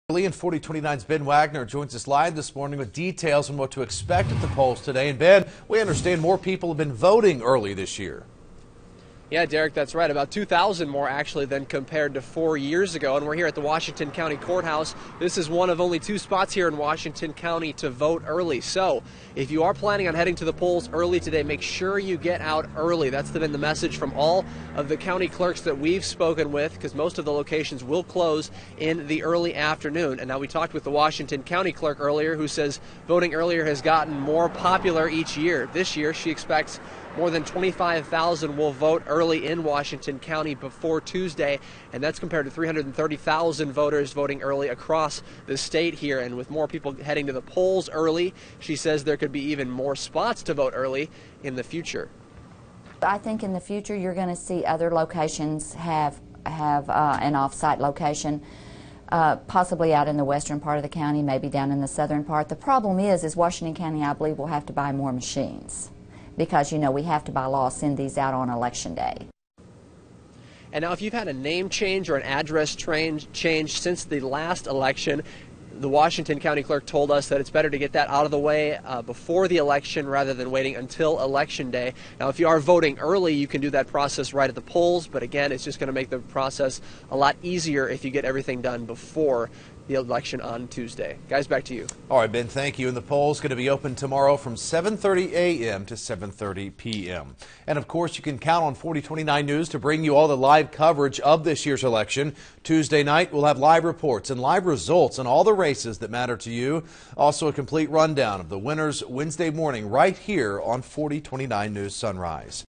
Monday is final day to Early Vote in Arkansas
Monday is final day to early vote in Arkansas. More people have early voted than in years past. Ben Wagner has details on what to expect at polls and for 40/29's 2012 election coverage.